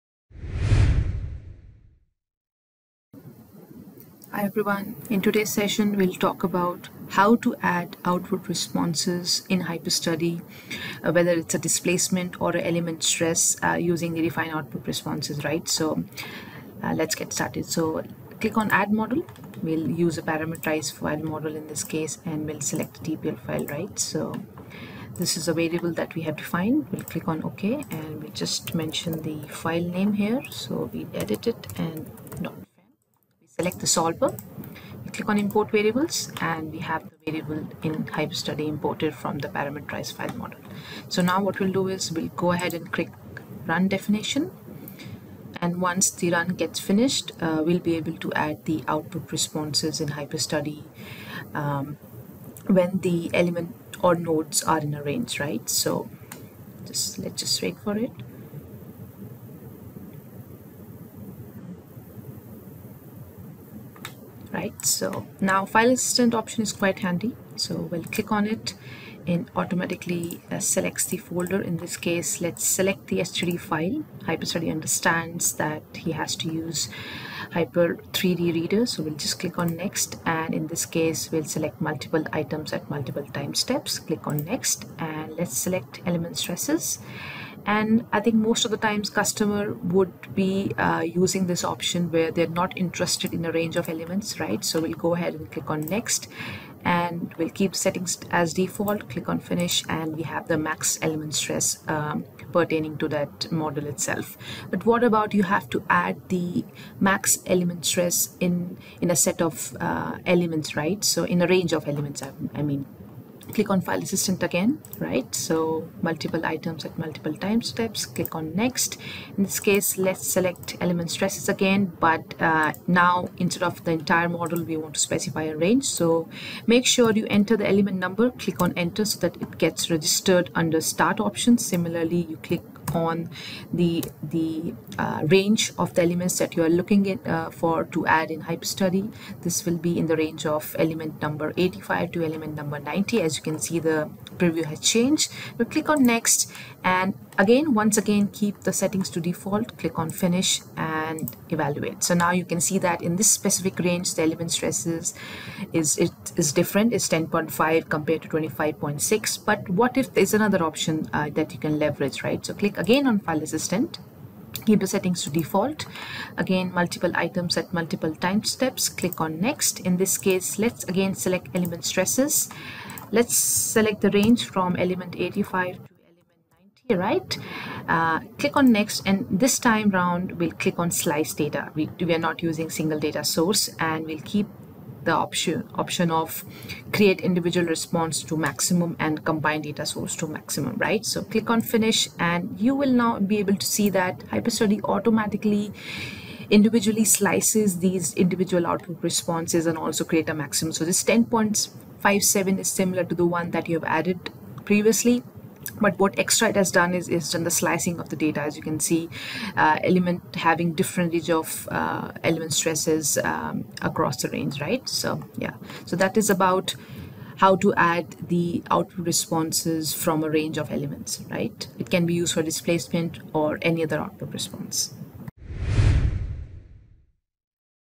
How to Define Output Responses over Elements Subset
In this video you will learn how to define output responses over subset of elements. For instance to extract Stress values on a specific range of elements of interest. An example of plate model solved with OptiStruct is used for the demonstration.